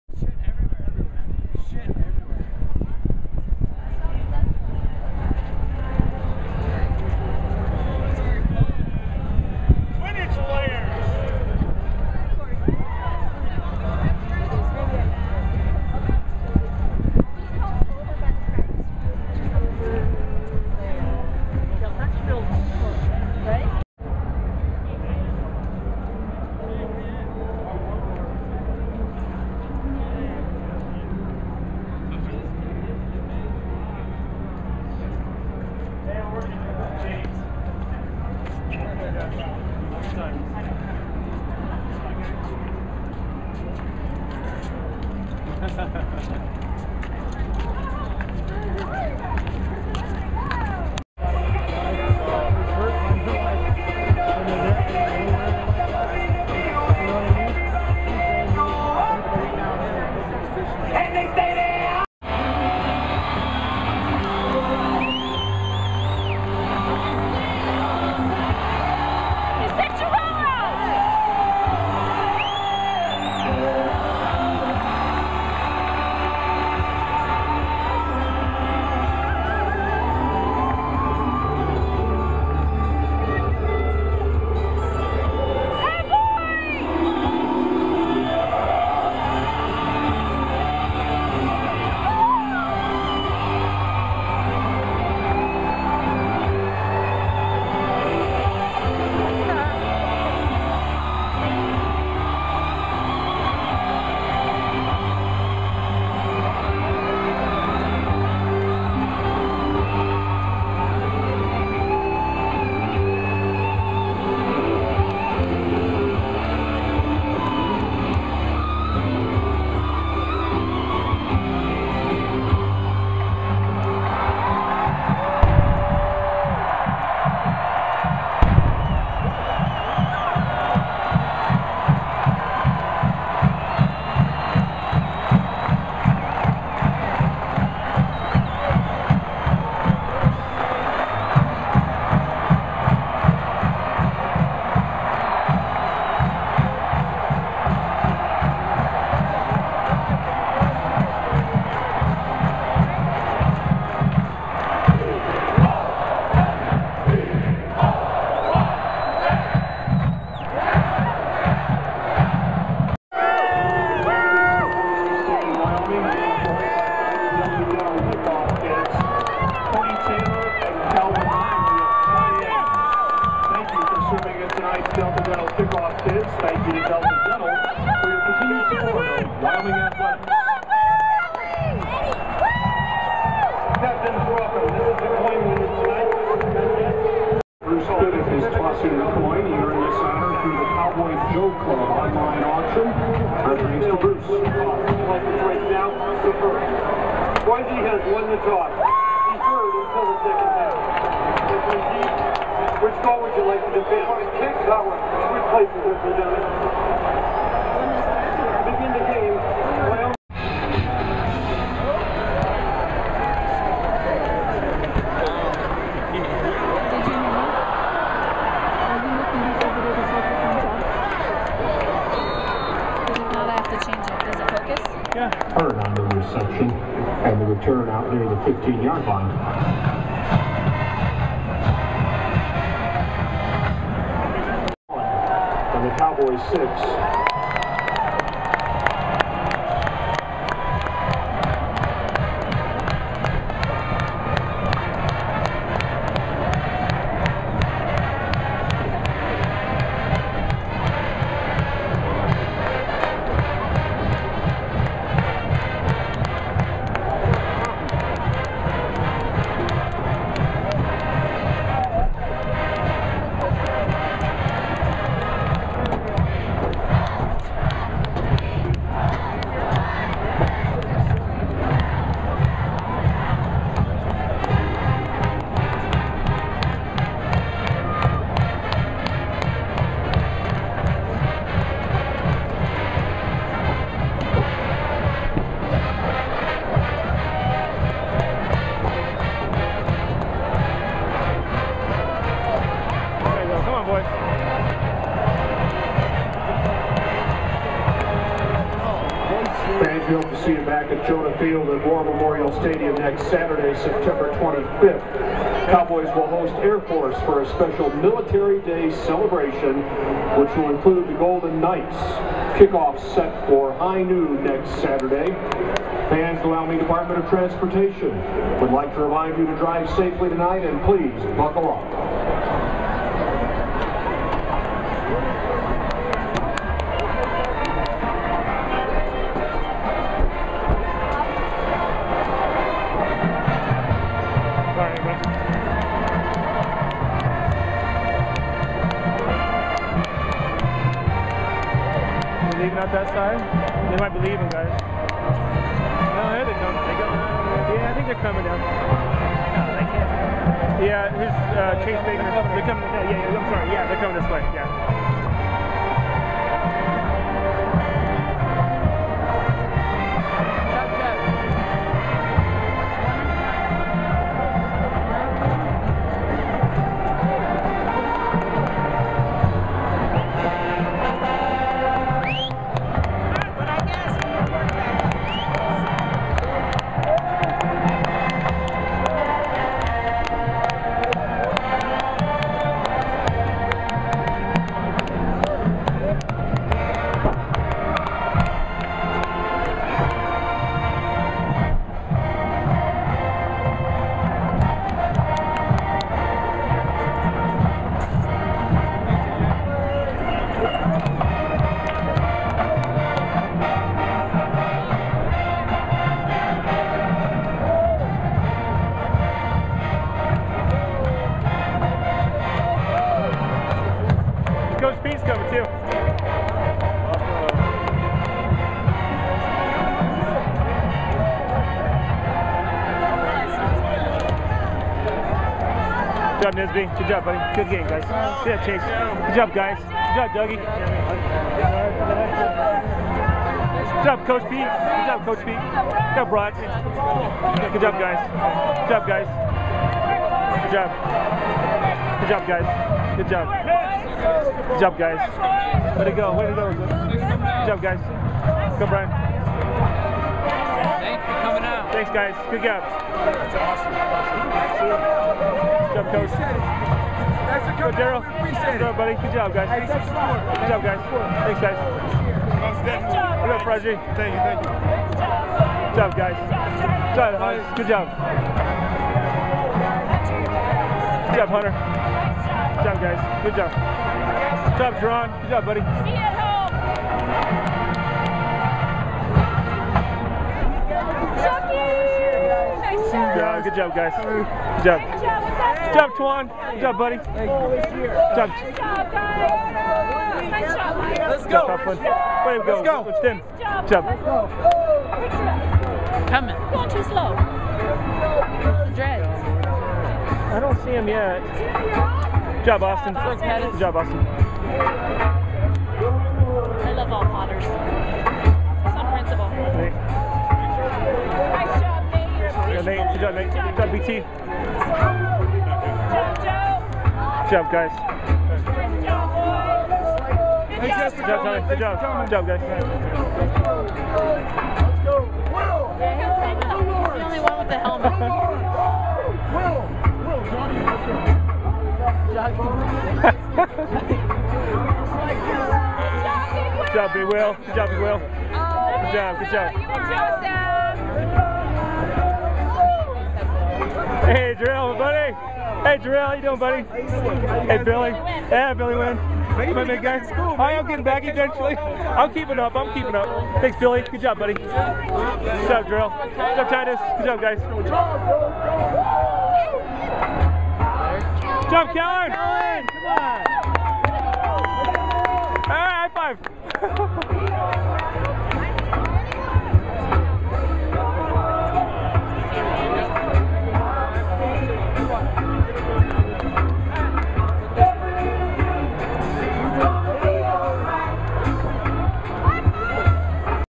Boise State vs Wyoming
Raw footage, Boise State vs Wyoming, Laramie WY September 18, 2010